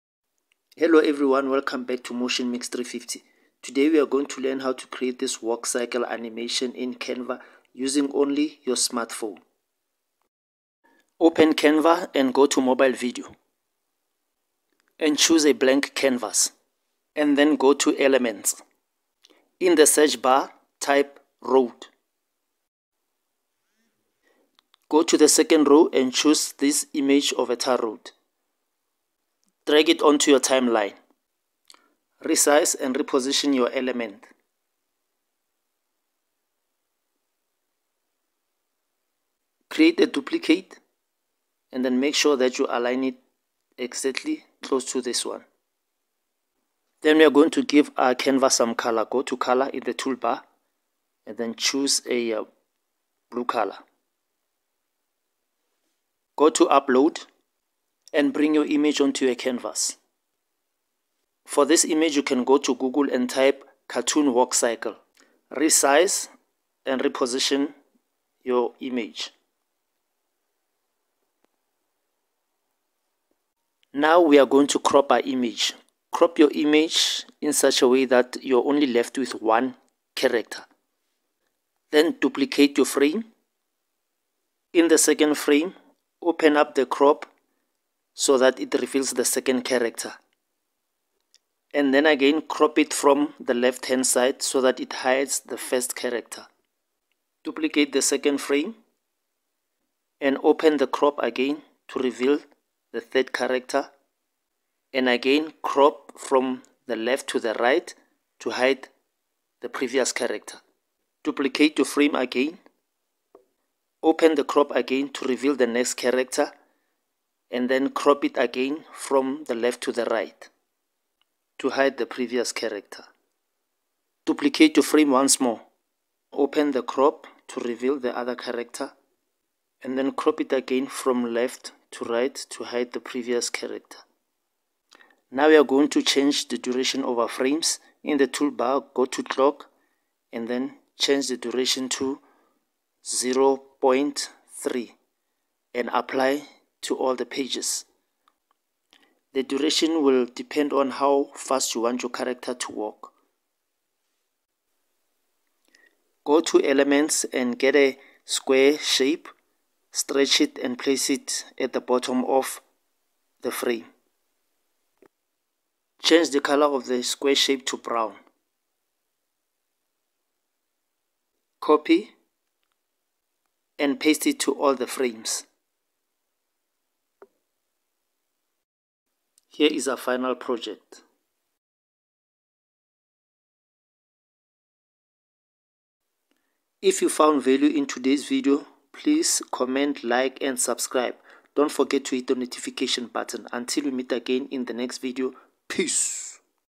How I created a Frame by Frame Animation in Canva
Today I'll show you how to create a simple Frame by Frame walk cycle animation in Canva even though Canva is not a Frame by Frame software/app.